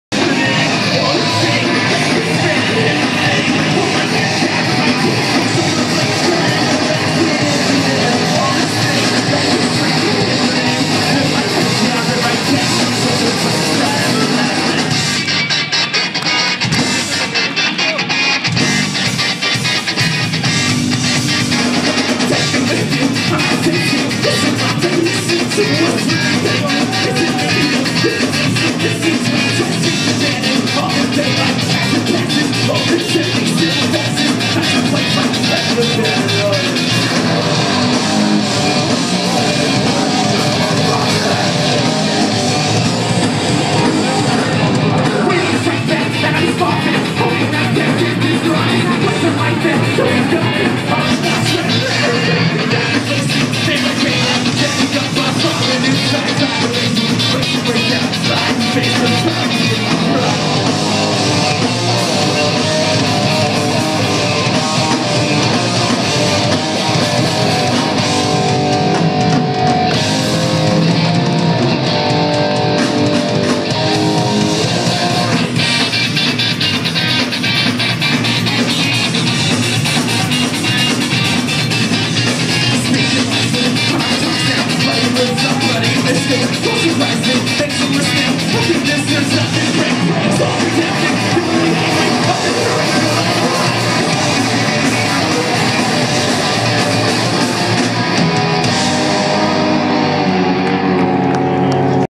Los Arambula
Los Arambula at the galaxy theater for the 8 Kalacas and Mafia Rusa cd release on 2/26/12. Skacore 818